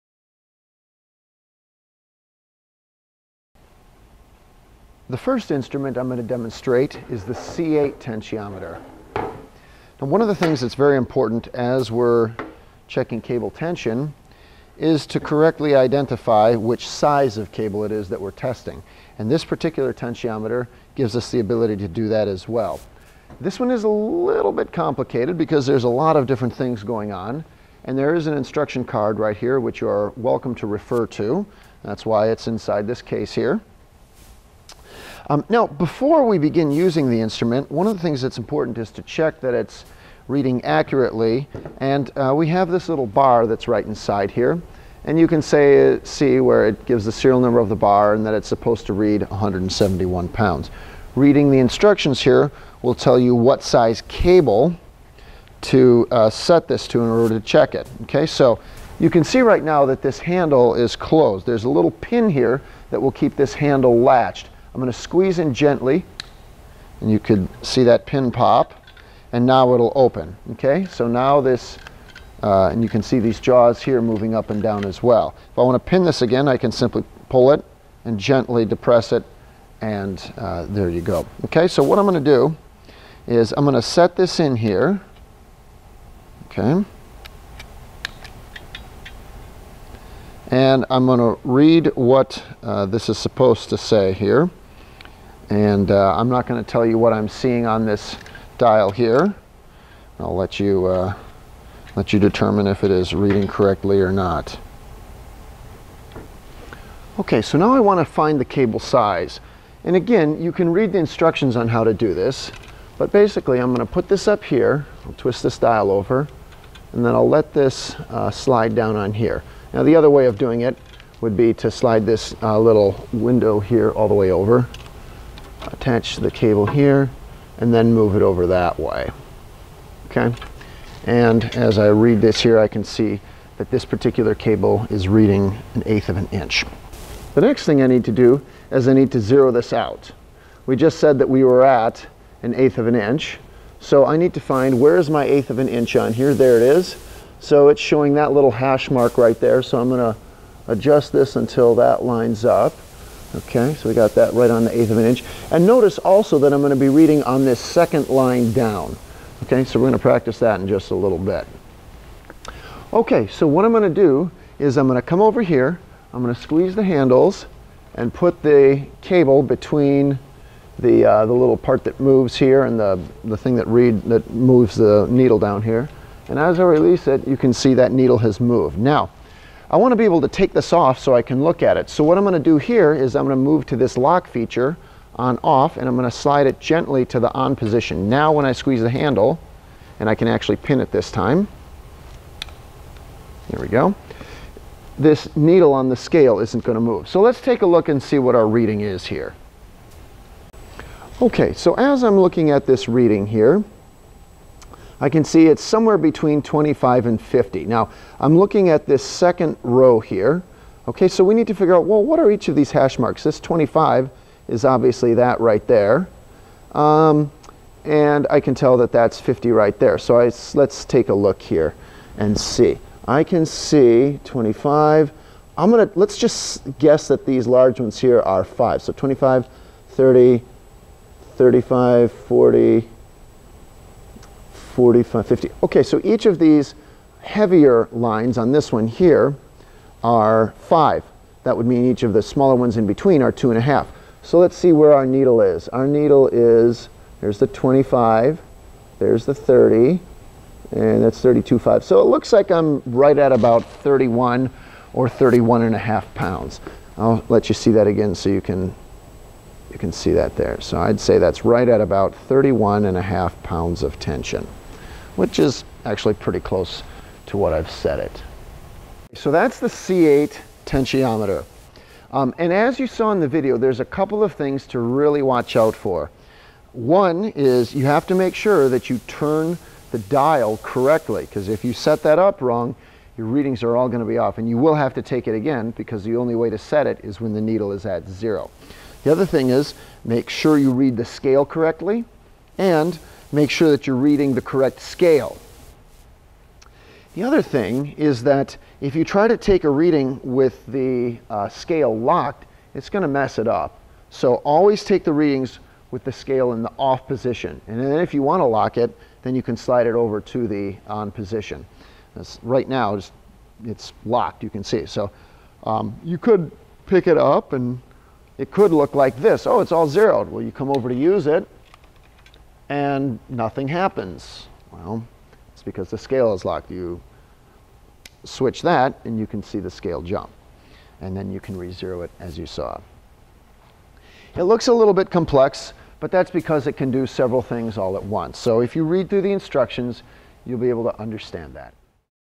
C-8 Tensiometer Demonstration
Demonstration of C-8 Tensiometer to measure cable tension and size.